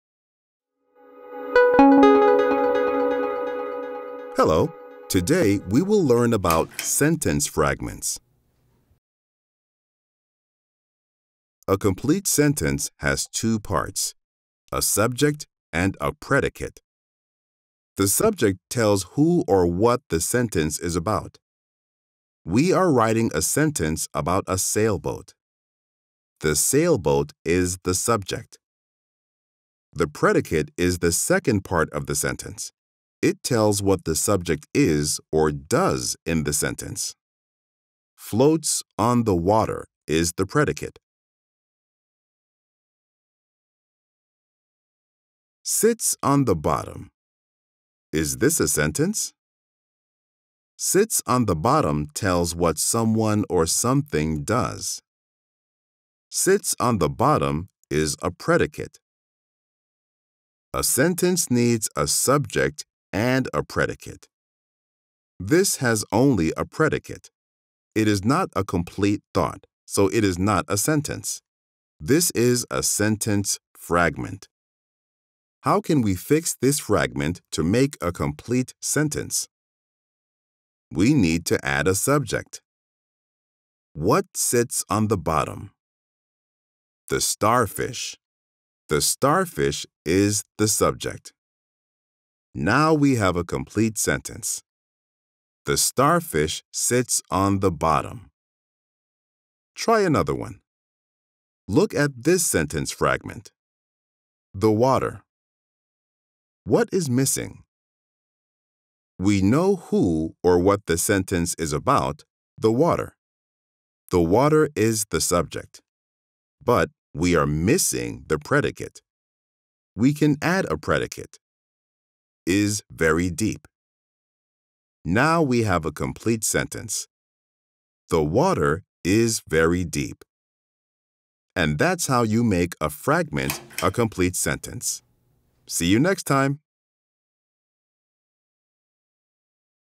Sentence Fragments
A sentence fragment is part of a sentence, or an incomplete sentence. Learn to identify and differentiate between whole sentences and sentence fragments.